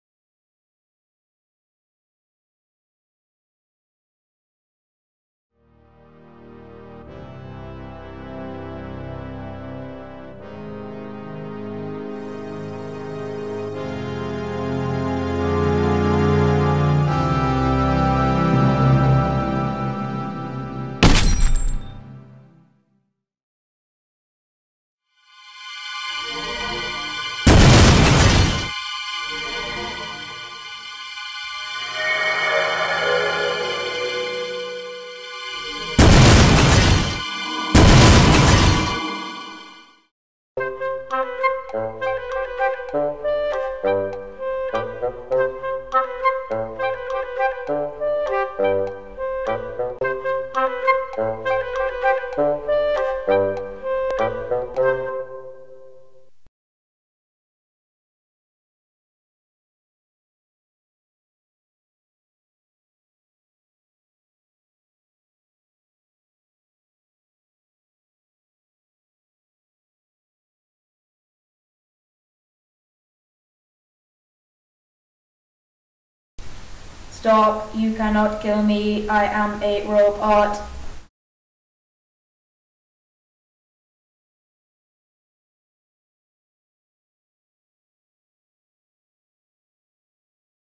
Turnip Starfish Animation February Half Term 2019
Had an great couple of days in half term making little animations with a cool group of young people in Cardiff.
Fancy having a go?